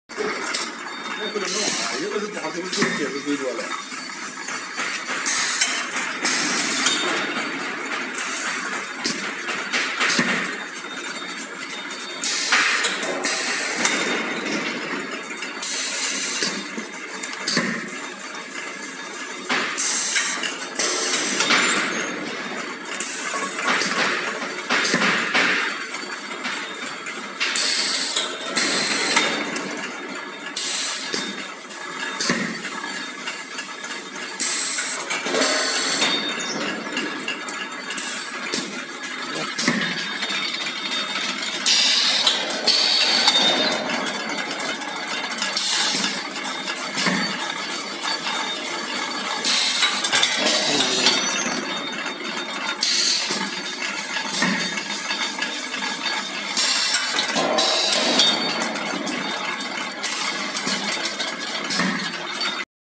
Ice Cream Waffle Cone Machine|Sugar Cones Machine
This waffle cone machine is suitable for making crispy waffle cone and sugar cones,which consists of 12 single-baking machine components,automatic cutting,the machine automatically after mature cone cake.